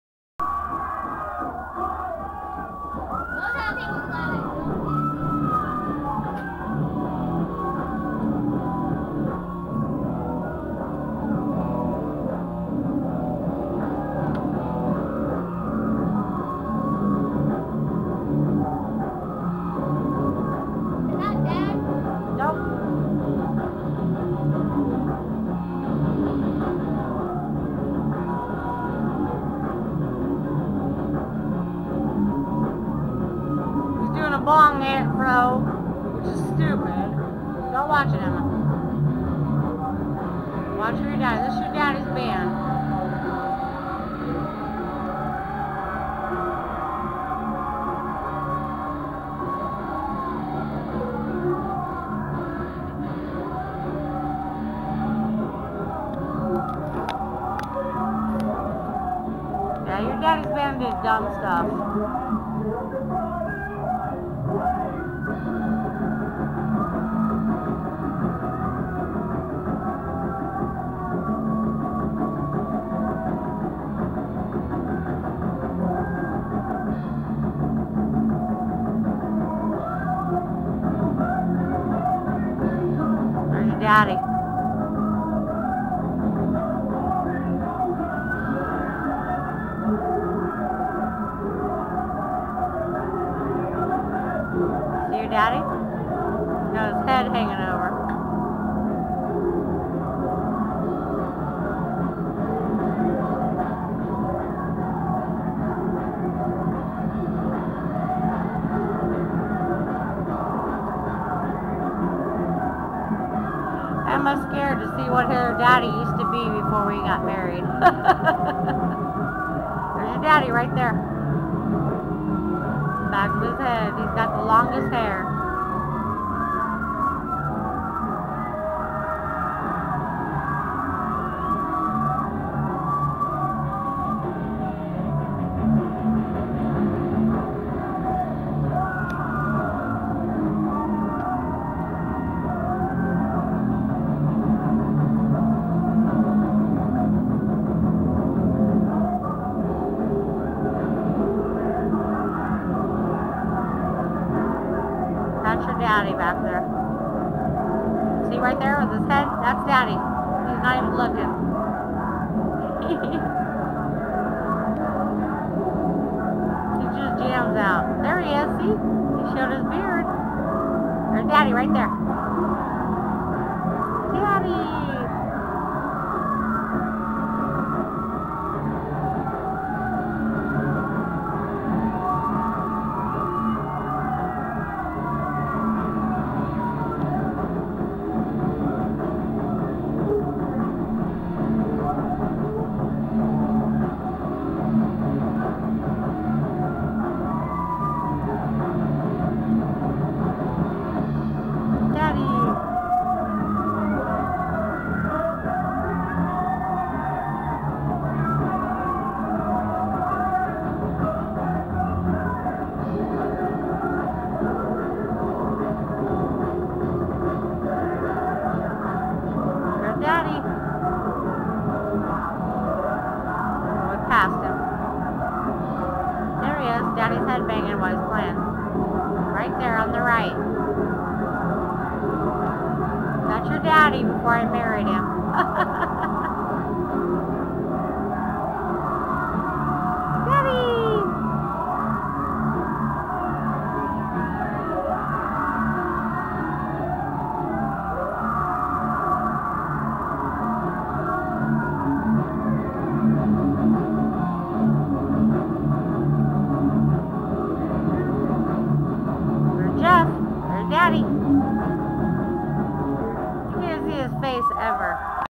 Nothing (The band daddy was in)
Bad recording but hey,,,that was decades ago!  His little girl don't recognize him! DesMoines 90's